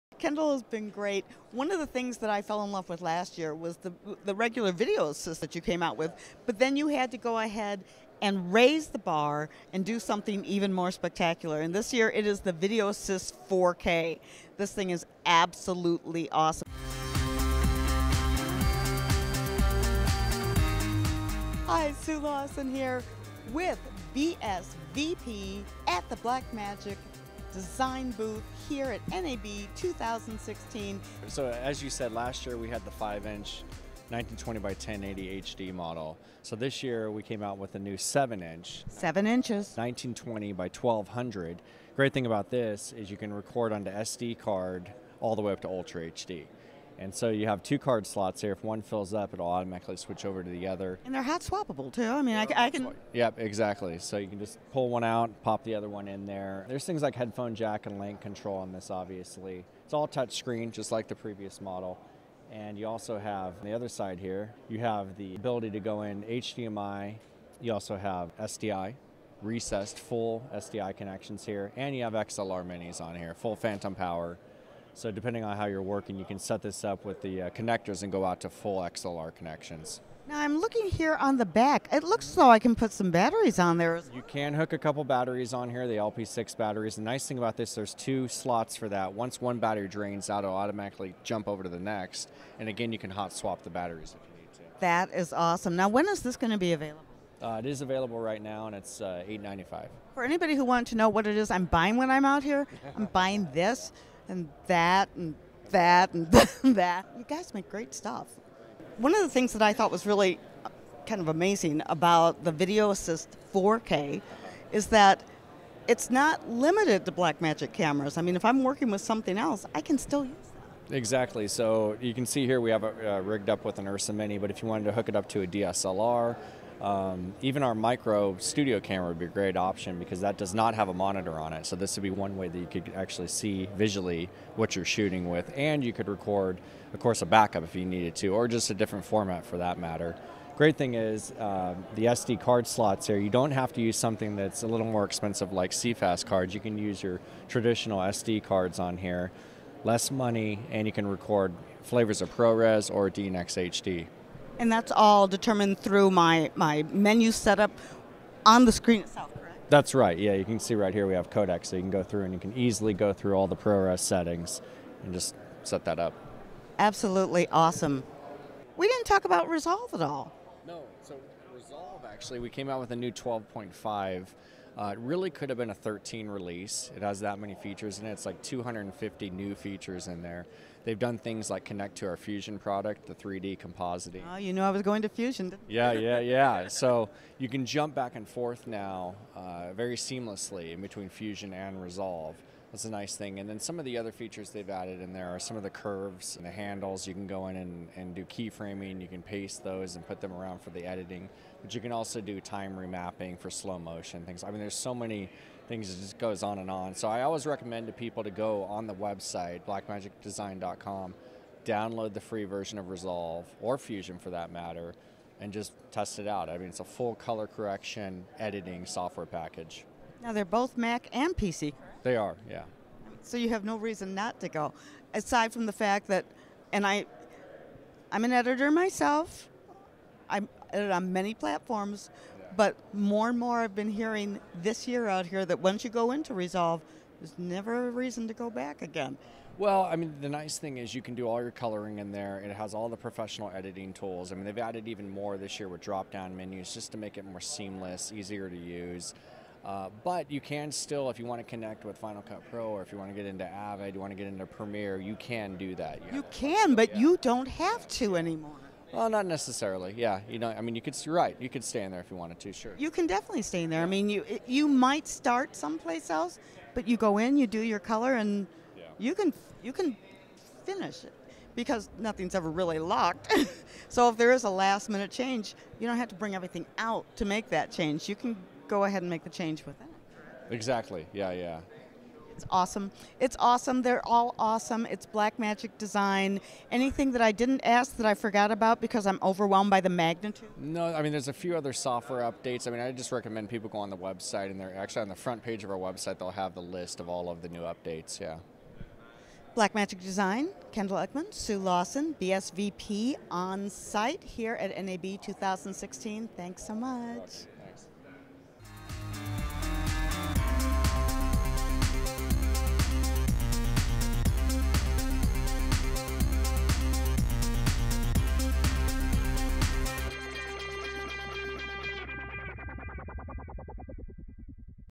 Blackmagic Video Assist Monitor/Recorder BSVP On-Site
BSVP On-Site: Tech Blackmagic Design Video Assist monitor

At NAB 2015, Blackmagic Design released their 5" video assist monitor/recorder 1080 HD model. At NAB 2016 they came out with the new 7 inch 1920 x 1200 model. This monitor/recorder will record onto SD card all the way up to Ultra HD. The Video Assist 4K has two card slots, so if one fills up it will switch over to the other.

The cards are hot-swappable so the you can pull out the full card and replace it with an empty card while the other slot is still recording.

The unit also comes with a headphone jack and LANC control, touch screen just like the previous model. You also have are HDMI and recessed SDI connections and 2 XLR connections.

2 LP six batteries slots, once one battery drains out automatically jump over the next battery.

The Video Assist is not limited to Blackmagic cameras.
You can hook it up to a DSLR even the Blackmagic Micro Studio camera would be a great option because that does not have a monitor on it. You could record a backup if you need to or just a different format for that matter. The great thing is the SD card slots, so you don't have to use something that's a little more expensive like C-Fast card. You can use her traditional SD cards on here, less money and you can record flavors of ProRes or DNX-HD.

Blackmagic also released DiVinci Resolve 12.5. Kendall Eckman stated that it could've been a 13 release because it has around 250 new features such as connecting it their fusion product and 3-D compositing. Both editing programs are available on the Blackmagic Design website.

So you can jump back-and-forth now very seamlessly in between Fusion and Resolve.  Curves and the handles are improved, cut and pasting of keyframes, cut and paste them around for the editing you can also do time remapping for slow motion so many things goes on anon so I always recommend people to go on the website design.com download the free version of resolve

The nice thing is you can do your coloring in Resolve, it has all the professional editing told him about it even more this year were drop-down menu to make it more seamless and easier to use. But you can still connect with Final Cut Pro, Premiere and Avid.

Because no production is ever really locked, so there is a last minute change, you don’t have to bring everything back to the original editing platform to make that change, you can go ahead make a change within Resolve.

Sue Lawson BSVP On-Site at NAB 2016

The Blackmagic Design Video Assist 4K is shipping now. Retail $895
For more information or a list of all of the new updates,